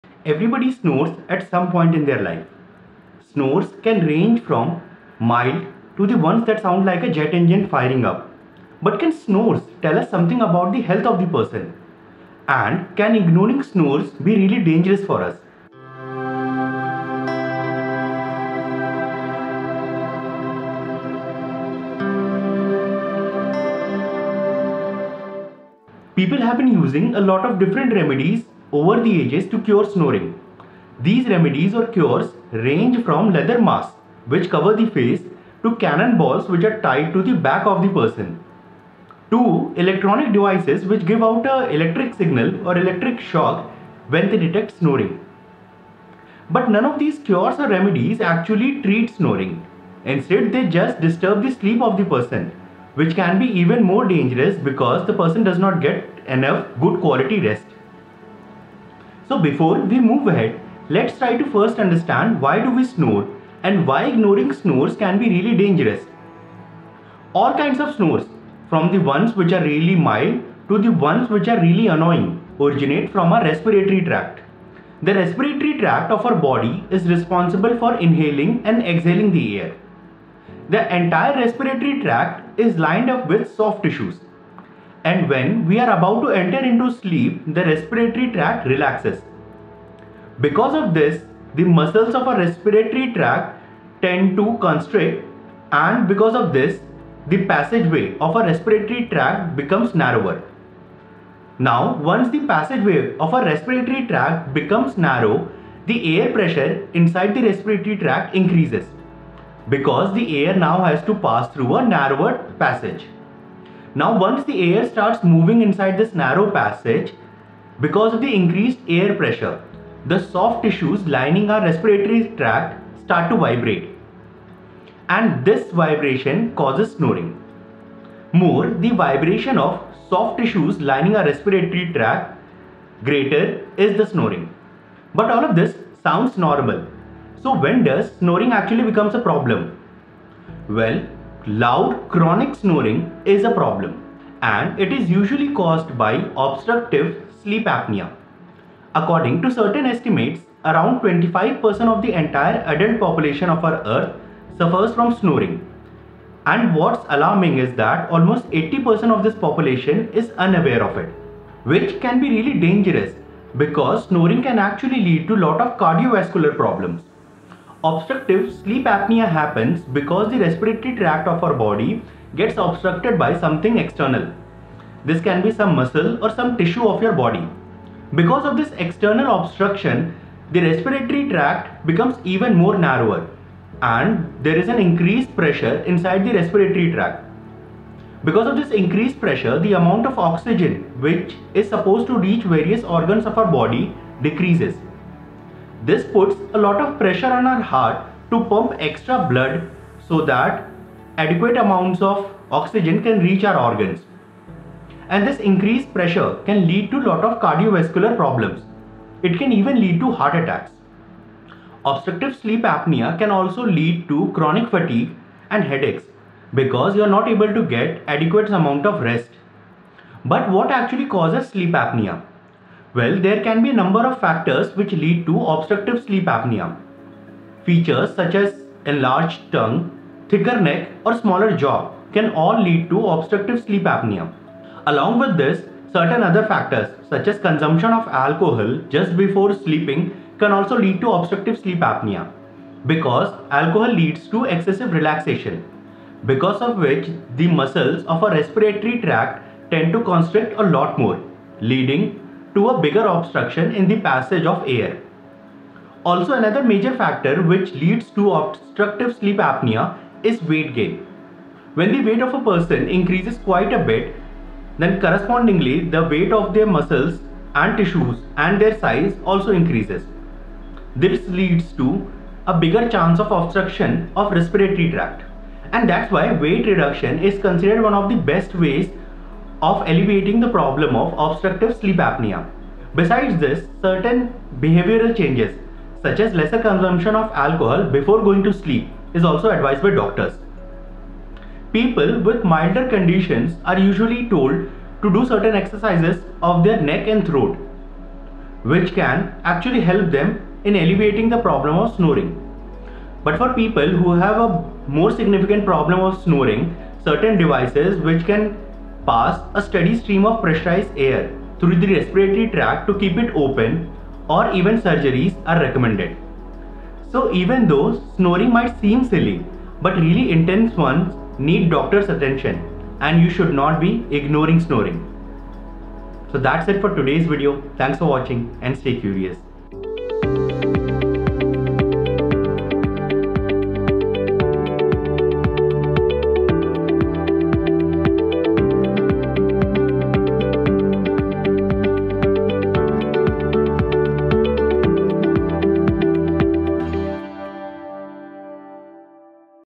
Why do we snore? | Is snoring a problem?
Every human being snore at some point in their life. Snores range from mild to extremely annoying. But snores tell a lot about a person's health and shouldn't be ignored.
Loud chronic snores are a result of major underlying issue with the respiratory tract of the body and should be given proper medical attention.